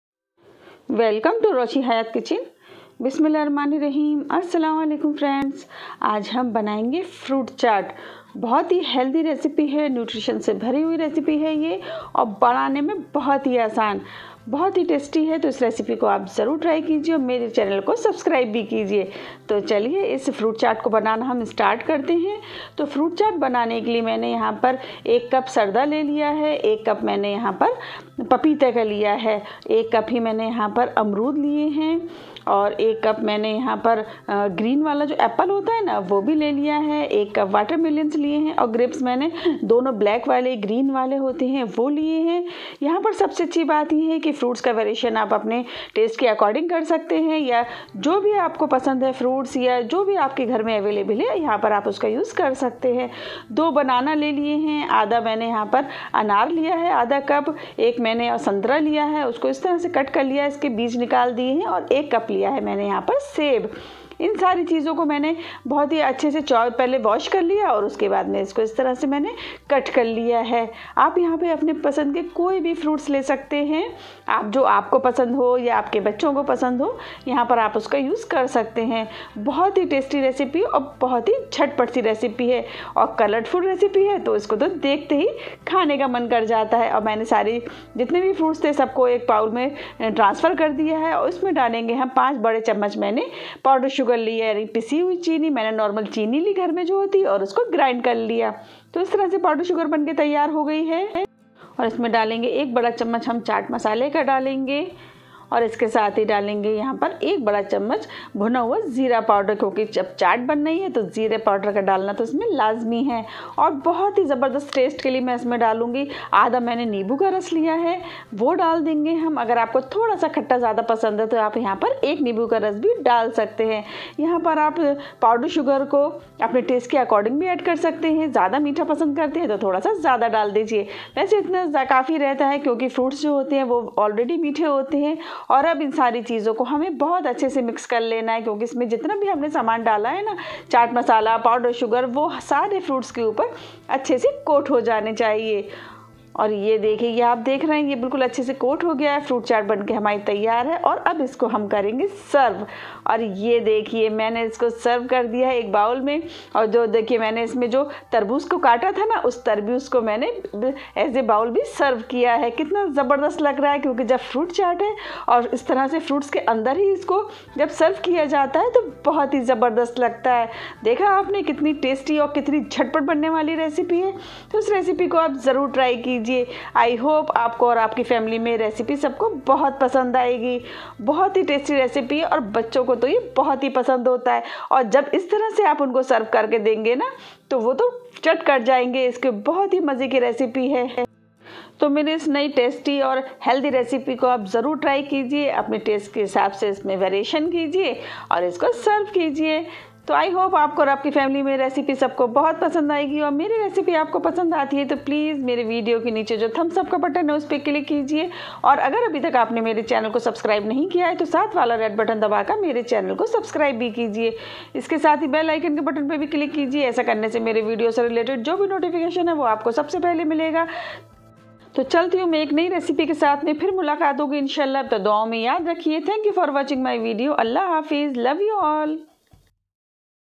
Fruit Chaat Recipe| फ्रूट चाट रेसिपी |Easy and Fast Street Style Fruit Chaat Masala Recipe 🌹♥️
Never miss any of your favourite Recipe update 😋😋😋

Aap hume Facebook or Instagram par bhi zarur follow Karein 😊🌹
Google 😊
cook with Roshi Hayat